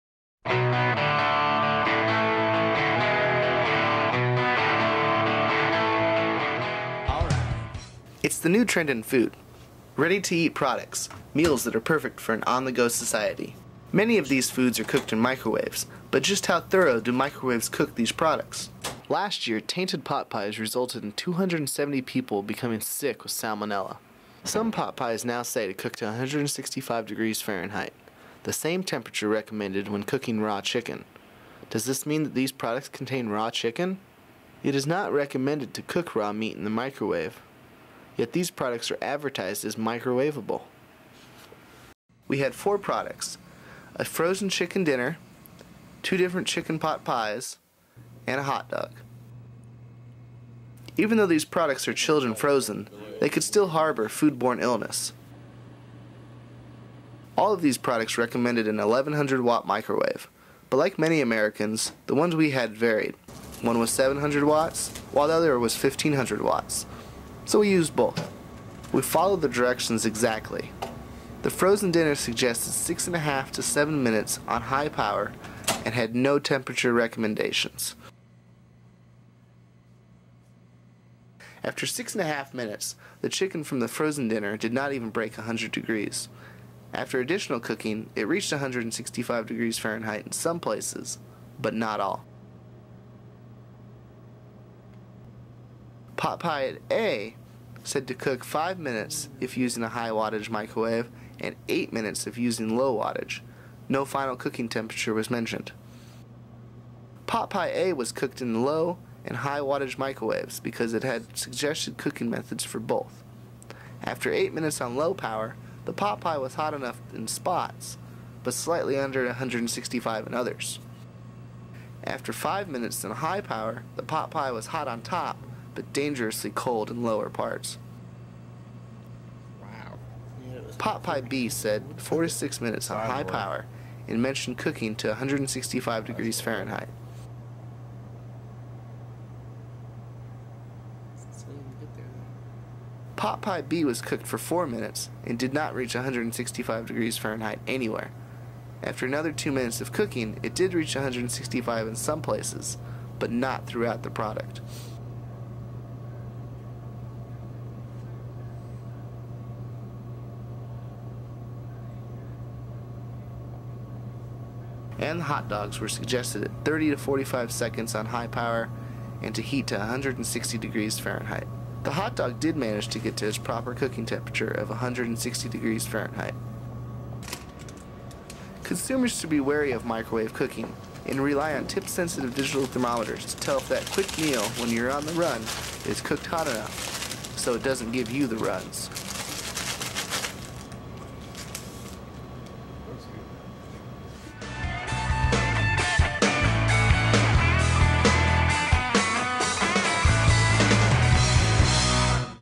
Microwaving pot pies and other frozen foods
A lot of products on the market today are microwavable. A recent outbreak of Salmonella in pot pies raised questions on if microwaves can properly cook frozen foods. The International Food Safety Network looks at some of these products to learn more.